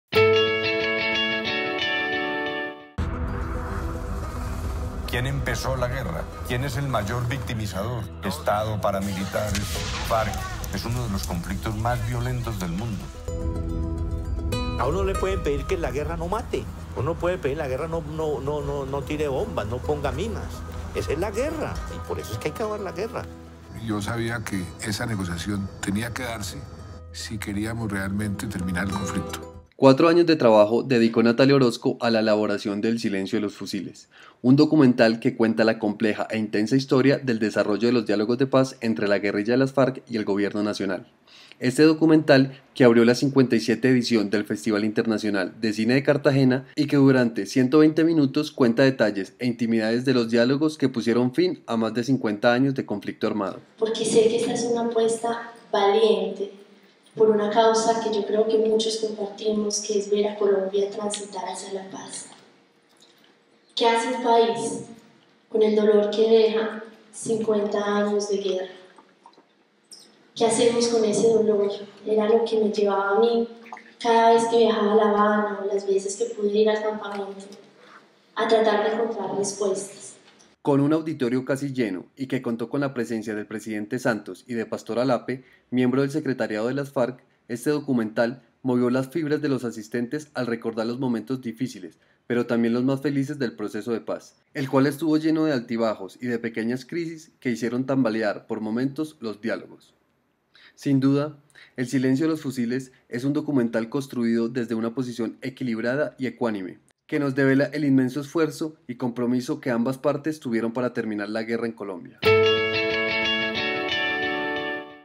"El Silencio de los Fusiles" una mirada a los diálogos de paz
4 años de trabajo dedicó Natalia Orozco a la elaboración de “El Silencio de los Fusiles”, un documental que cuenta la compleja e intensa historia del desarrollo de los diálogos de paz entre la guerrilla de las FARC y el Gobierno nacional.
Este documental que abrió la 57 edición de Festival Internacional de Cine de Cartagena, y que durante 120 minutos cuenta detalles e intimidades de los diálogos que pusieron fin a mas de 50 años de conflicto armado.

En un auditorio casi lleno y que contó con la presencia del Presidente Juan Manuel Santos y Pastor Alape, miembro del secretariado de las FARC, éste documental movió las fibras de los asistentes al recordar los momentos difíciles pero también los mas felices del proceso de paz, el cual estuvo lleno de altibajos y de pequeñas crisis que hicieron por momentos tambalear los diálogos.

Sin duda “El Silencio de los Fusiles” es un documental construido desde una posición equilibrada y ecuánime  que nos devela el inmenso esfuerzo y compromiso que ambas partes tuvieron para terminar la guerra en Colombia.